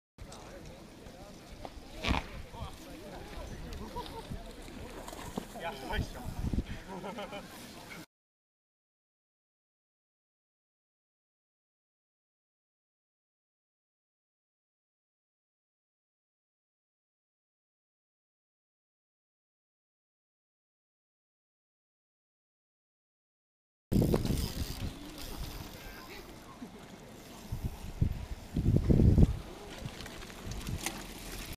120fps撮影で癖チェック
バンク→ジャンプまでの間でコギが入ってる。
ジャンプの後のバンク出口で右手を握り直してるのを確認。
自分では気づかなかったw